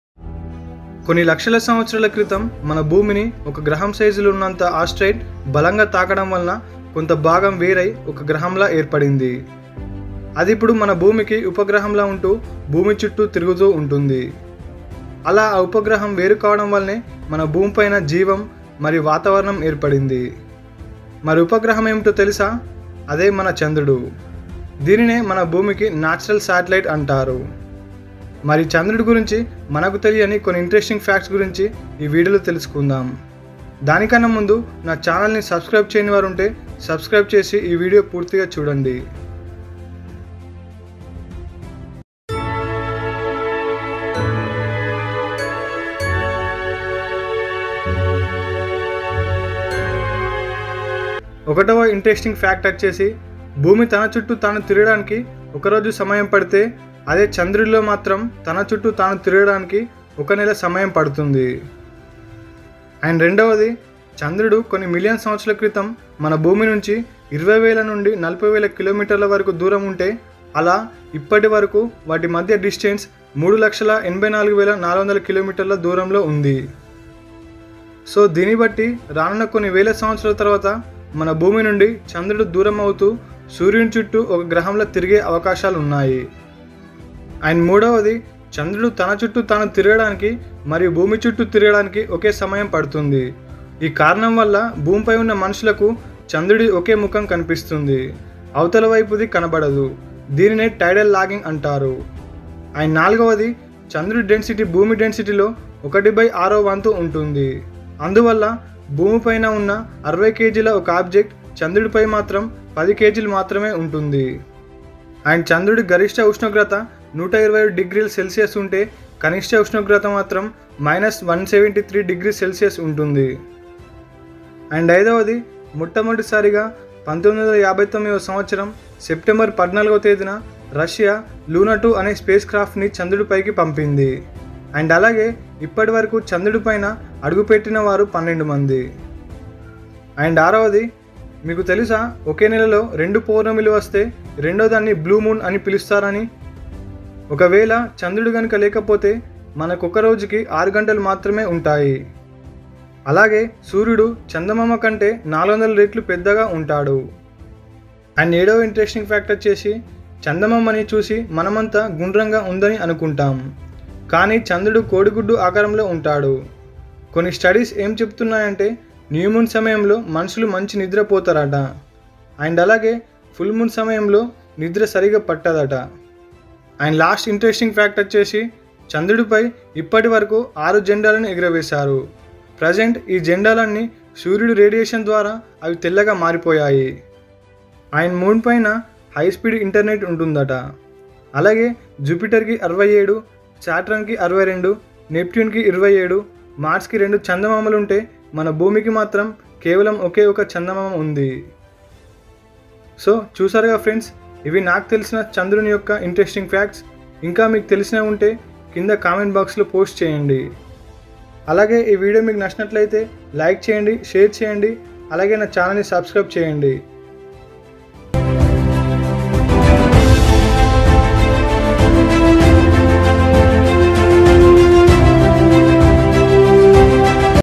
Interesting facts about moon and How the moon was formed..? | In Telugu by MN techtube (Nagaraj.m)
Nagaraju Marupaka
By MN TECHTUBE
(M.Nagaraju)

The Moon is Earth's only proper natural satellite. At one-quarter the diameter of Earth, it is the largest natural satellite in the Solar System relative to the size of its planet, and the fifth largest satellite in the Solar System overall.

Distance to Earth: 384,400 km
Gravity: 1.62 m/s²
Radius: 1,737.1 km
Age: 4.53 billion years
Alternative names: Luna; Selene (poetic); Cynthia (poetic)
Surface area: 3.793×107 km2; (0.074 of Earth's)

(Thank you Anudeep katha)